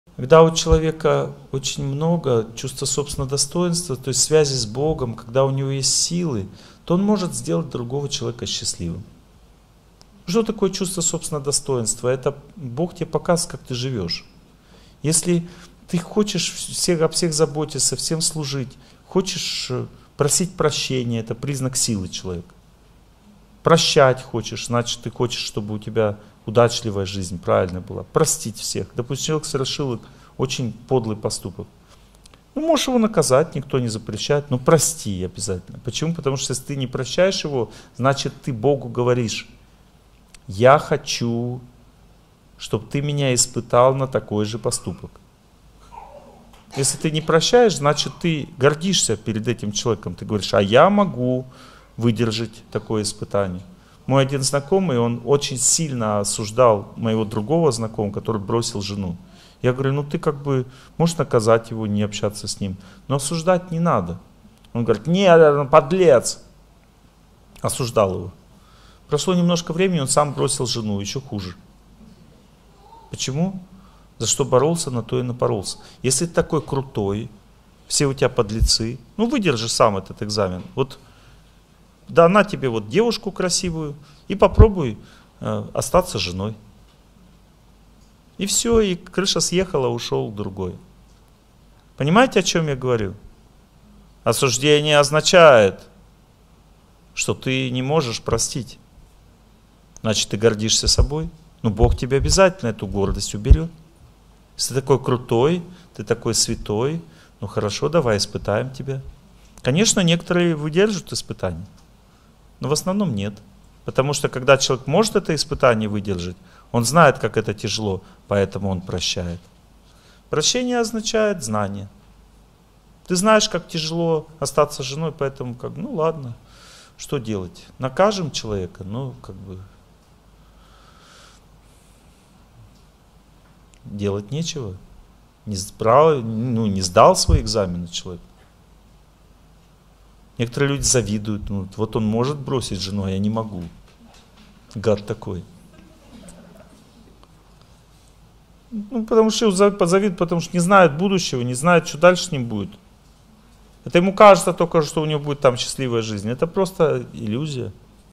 Торсунов О.Г. О прощении
В этом видео Олег Геннадьевич Торсунов говорит о прощении
Из семинара ""Чувство собственного достоинства" Бишкек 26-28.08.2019